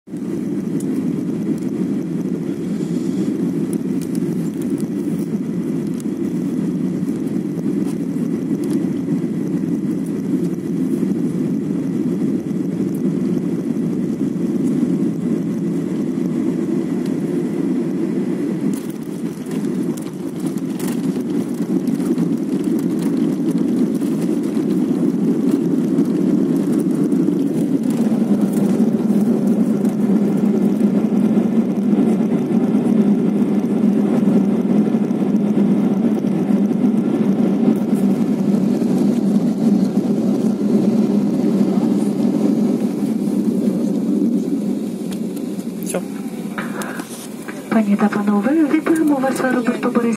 Shooting on the Xiaomi Mi5 - Landing in Borispol
An example of shooting on the Xiaomi Mi5 smartphone.
Landing in Borispol